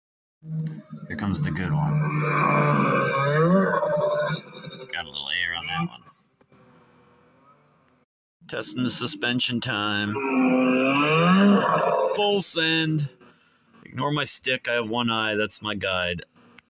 neighbor kid your turn send it
Well fellas felletes they thems and whoever takes the time to listen to this beauty and watch!
  Last night the neighbor kid got some air... looked great but!!!
Ya know I had show him how its done!!! I hope you all enjoy this short bit of footage thing is a screamer!

Just having a good time and sending it tonight!!!